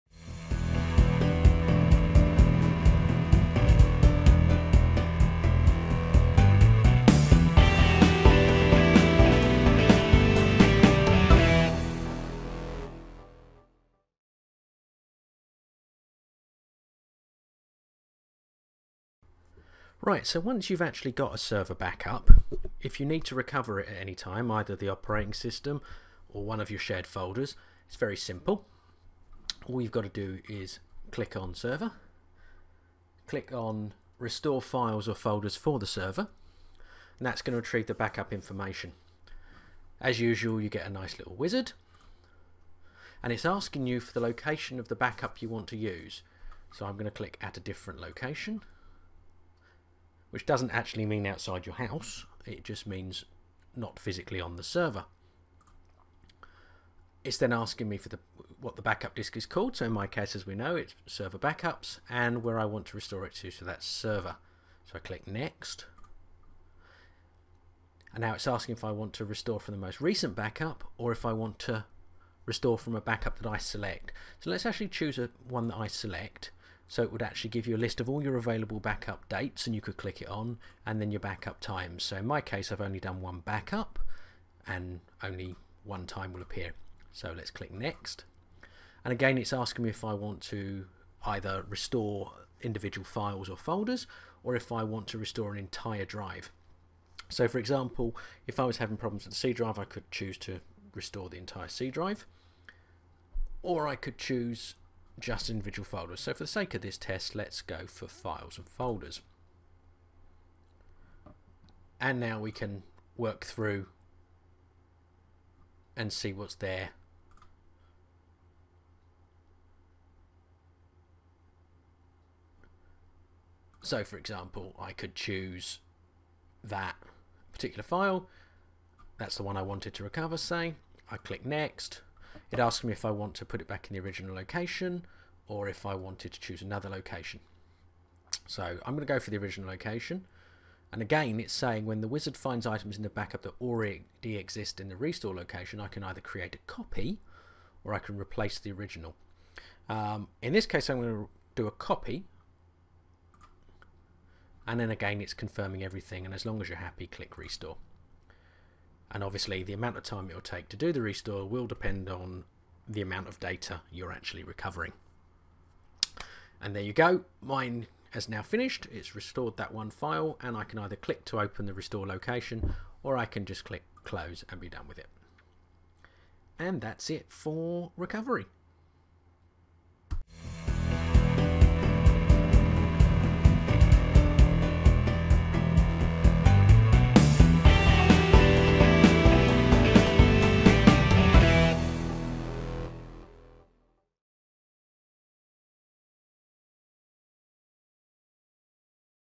WHS 2011 Server and Folder Recovery.mp4
UWHS : Using Windows Home Server.com takes a look at the new Server and Folder Recovery functionality included in Windows Home Server 2011 - formally Vail.